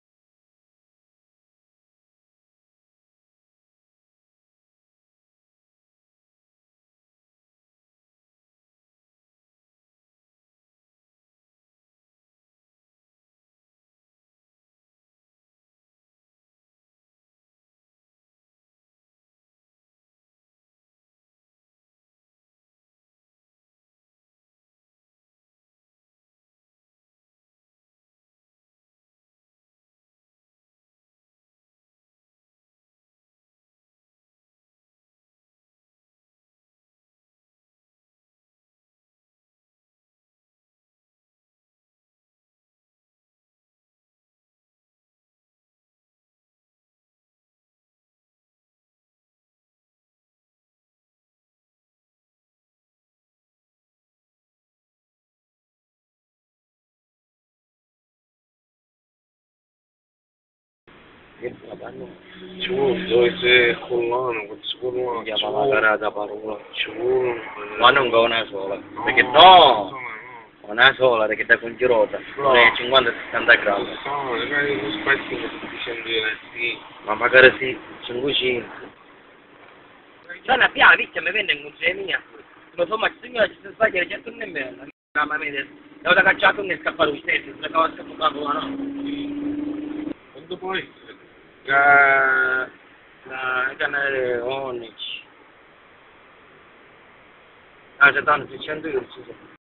Mafia, blitz "Adernò": le intercettazioni prima degli arresti tra Biancavilla e Adrano
VAI AL SITO
►Biancavilla Oggi